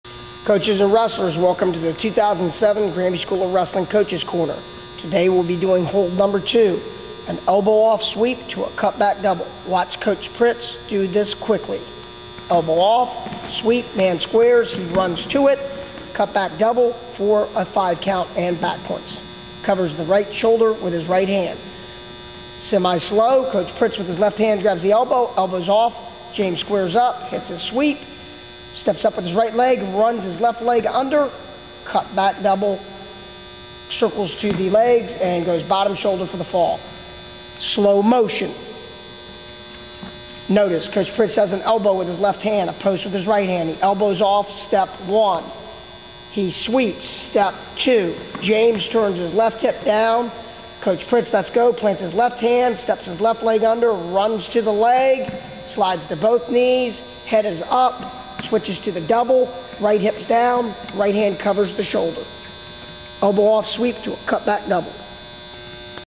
Granby School of Wrestling Technique Series #2
Steve Martin's Granby School of Wrestling demonstrates the Sweep Cut Back